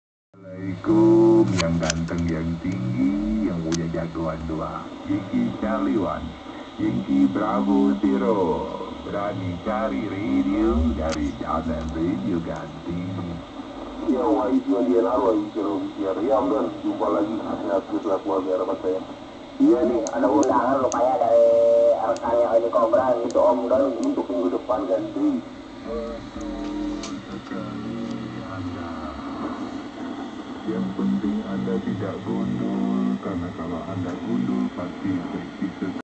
80m SSB Receiver using TA7358 (LA1185)
using  mixer chips TA7358 (LA1185) for ssb receiver  and get surprise for their performance